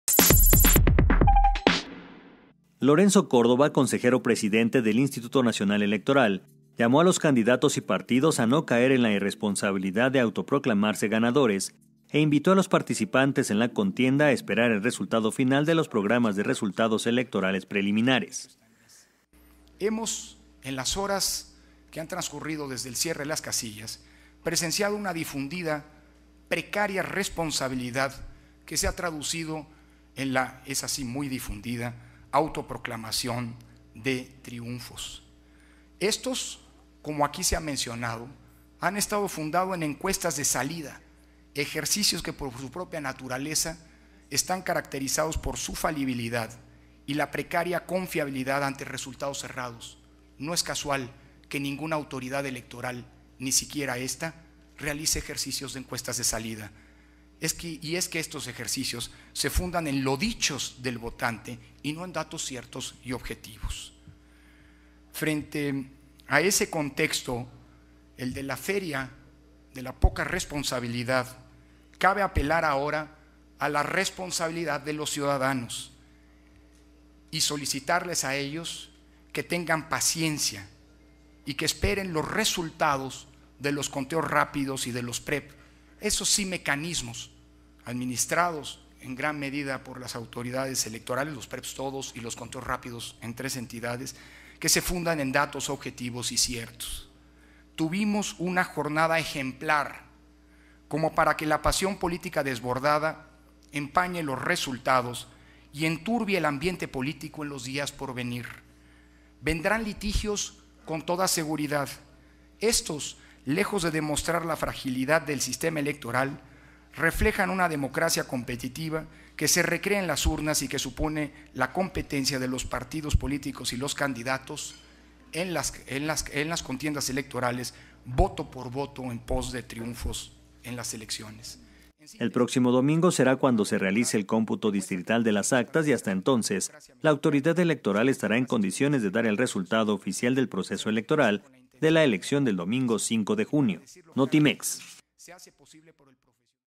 Pide INE evitar autoproclamarse ganadores
Lorenzo Córdova, Consejero Presidente del INE invitó a los participantes en la contienda a esperar el resultado final del PREP.